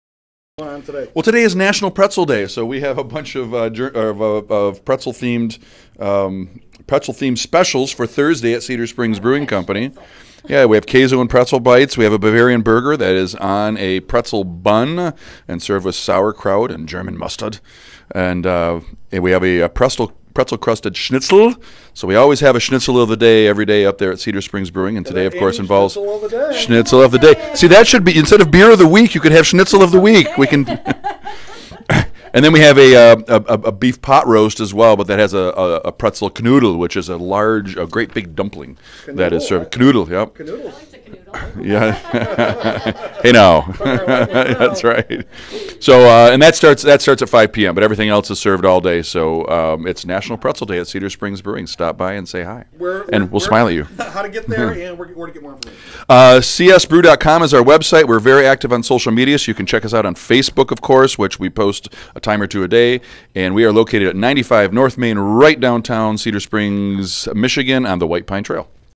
National Pretzel Day at Cedar Springs Brewing Company 2018
David Ringler of Cedar Springs Brewing Company runs down some specials for National Pretzel Day 2018.